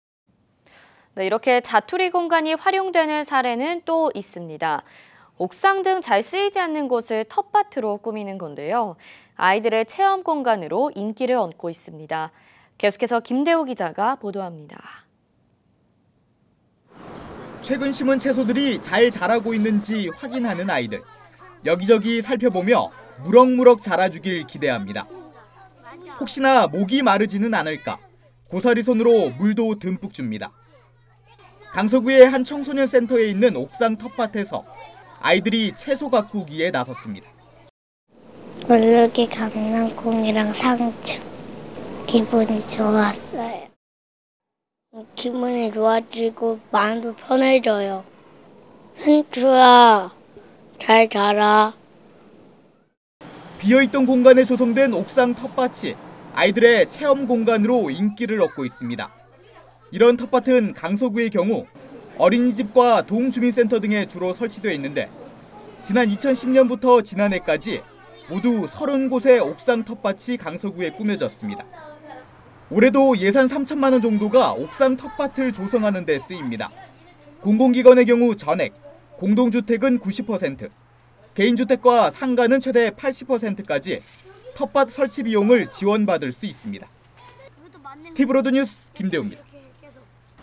건물 자투리 공간 '옥상'...텃밭 활용 '인기'_티브로드서울
[티브로드 김대우기자]

[앵커멘트]
자투리 공간이 활용되는 사례는 또 있습니다.
옥상 등 잘 쓰이지 않는 곳을
텃밭으로 꾸미는 건데요.
아이들의 체험공간으로 인기를 얻고 있습니다.
계속해서 김대우 기자가 보도합니다.

[기사내용]
최근 심은 채소들이
잘 자라고 있는지 확인하는 아이들.

여기 저기 살펴보며
무럭무럭 자라주길 기대합니다.

혹시나 목이 마르지는 않을까
고사리손으로 물도 듬뿍 줍니다.

강서구의 한 청소년센터에 있는 옥상텃밭에서
아이들이 채소 가꾸기에 나섰습니다.

이시온 / 국제청소년센터 유아스포츠단
(강낭콩이랑 상추를 (심었어요.) 기분이 좋았어요.)

김기율 / 국제청소년센터 유아스포츠단
((채소를 보니까) 기분이 좋아지고
마음이 편해져요. 상추야 잘자라.)

비어 있던 공간에 조성된 옥상텃밭이

이런 텃밭은 강서구의 경우
어린이집과 동주민센터 등에 주로 설치돼 있는데,
지난 2010년부터 지난해까지
모두 30곳의 옥상텃밭이 강서구에 꾸며졌습니다.

올해도 예산 3천만 원 정도가
옥상텃밭을 조성하는 데 쓰입니다.

공공기관의 경우 전액, 공동주택은 90%,
개인주택과 상가는 최대 80%까지
텃밭 설치비용을 지원받을 수 있습니다.

티브로드 뉴스 김대우입니다.

(촬영/편집 - 김환 기자)